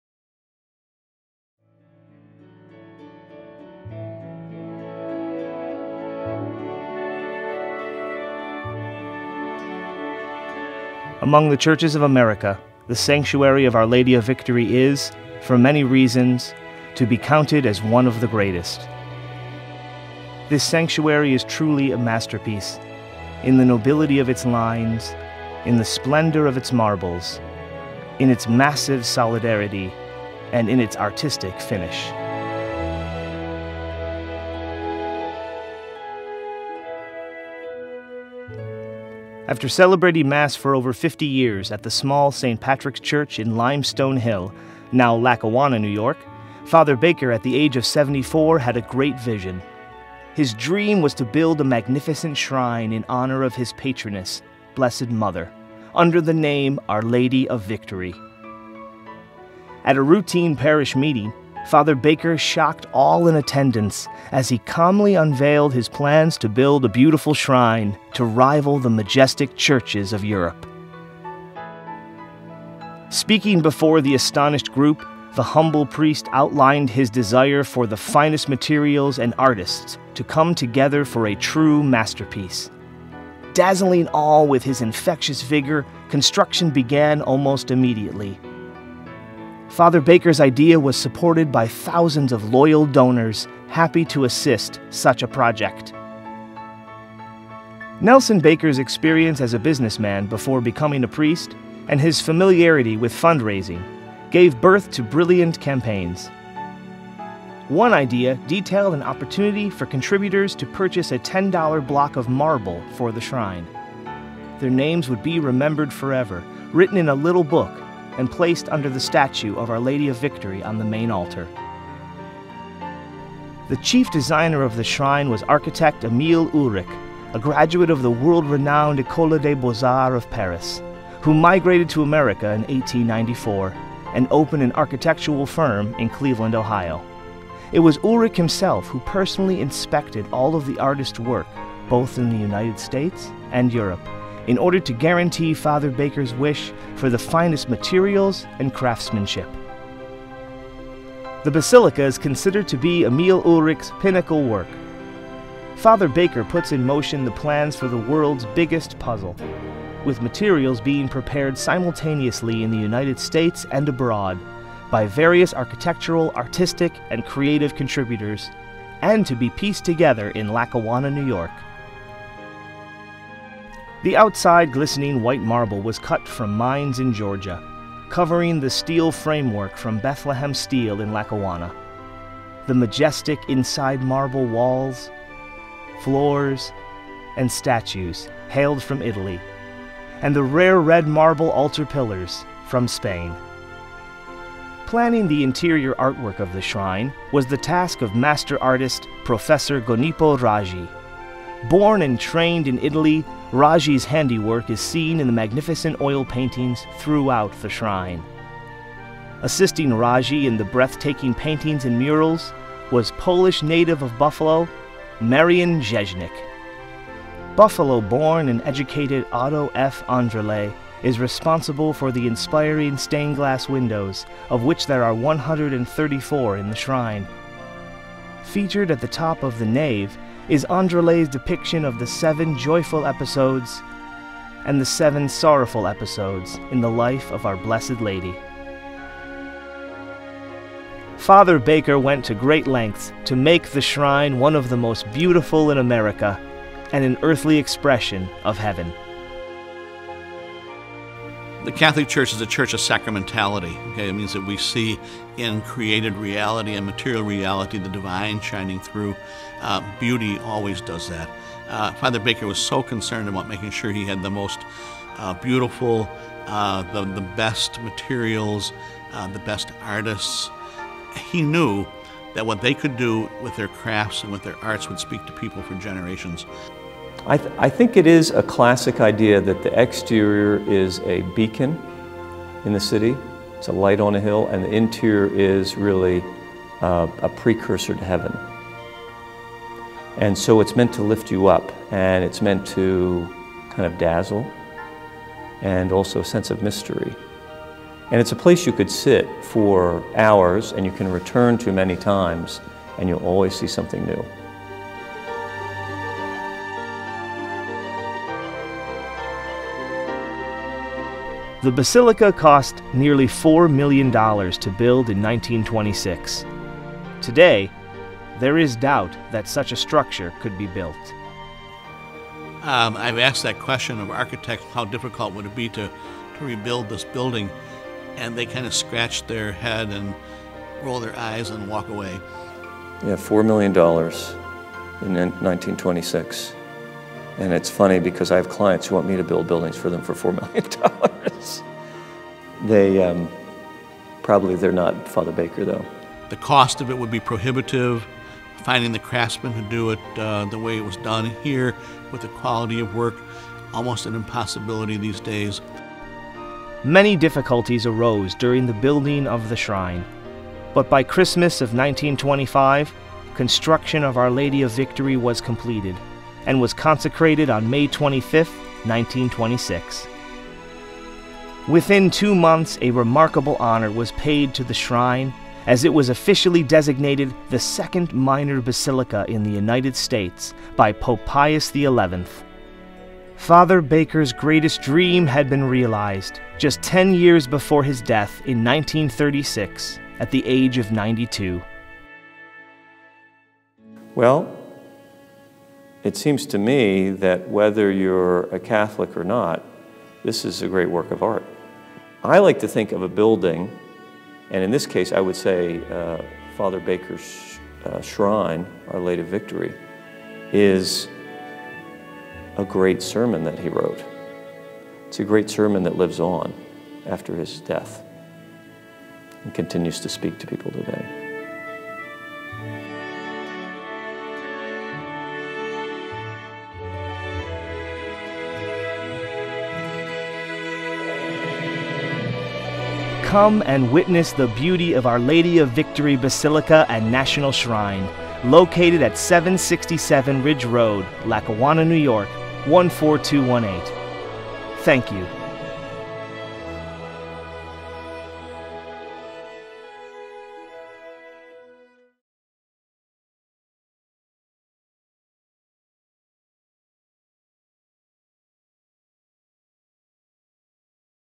Our Lady of Victory Basilica - National Shrine
Video about the building of Our Lady of Victory Minor Basilica and National Shrine in Lackawanna, New York. Built by Msgr. Nelson Baker and architect Emile Ulhrich 1926